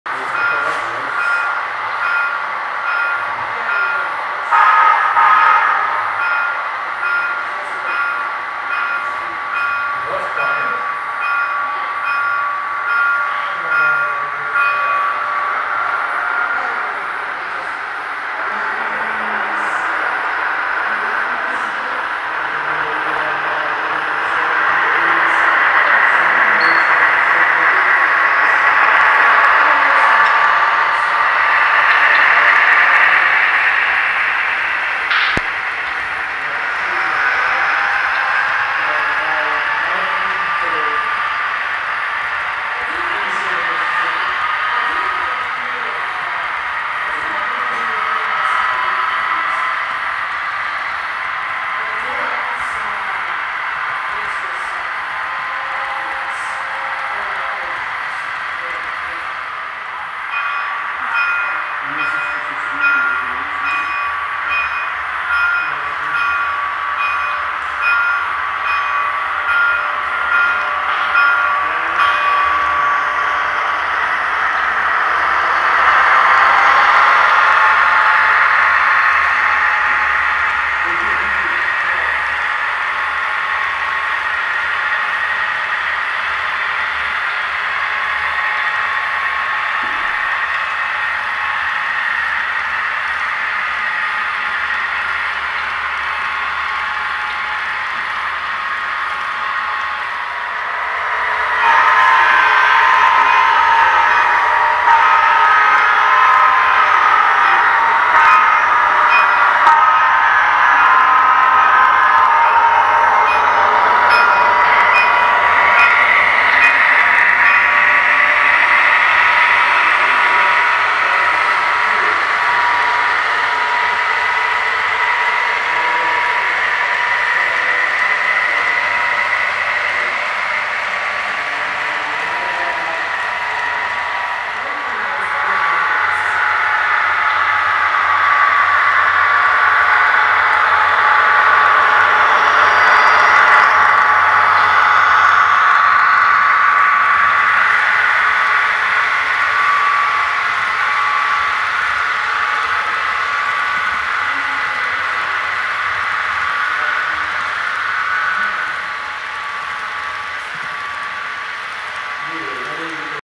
ProtoLoads on the DUSMRE.
A Conrail freight train pulls a string of cars from ProtoLoads.  The loads for the freight cars include steel rounds, truck frames, utility poles, drill pipe, and a gondola with tie rack extensions.  From the operating session held on September 1 2018.